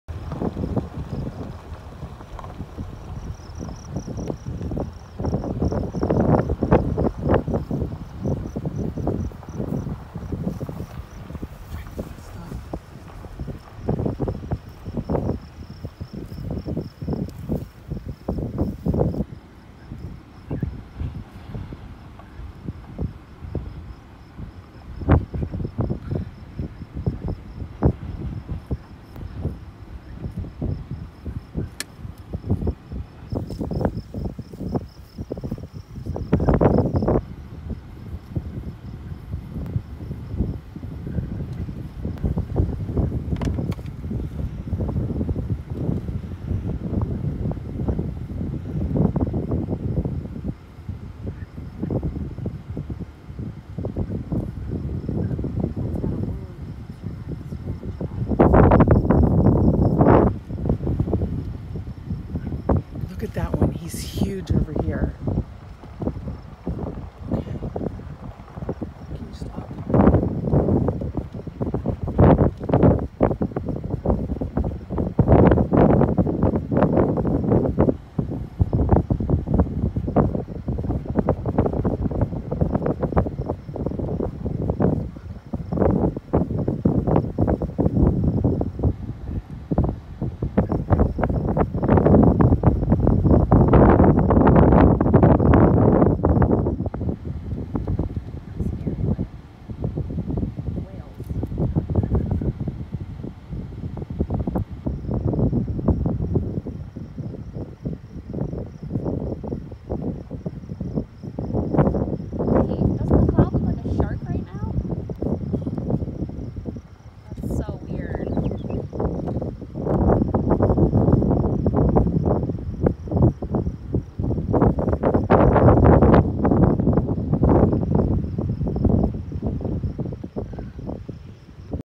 Buffalo in the Badlands South Dakota
But doesn't that cloud look like a shark?

We turned around to go back to see the bison, but they were no where to be seen.  Within 5 minutes they were gone, somewhere in the prairie.   Super cool that we got to see them close up.

Bison in the Badlands July 2022